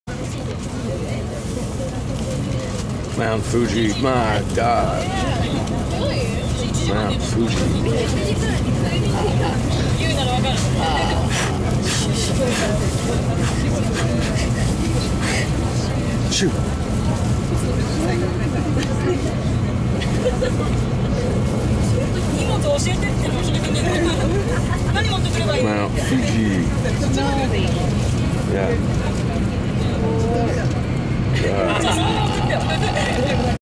Mt Fuji from Shinkansen
Mount Fuji can be seen briefly while processing the Japanese landscape via Bullet Train.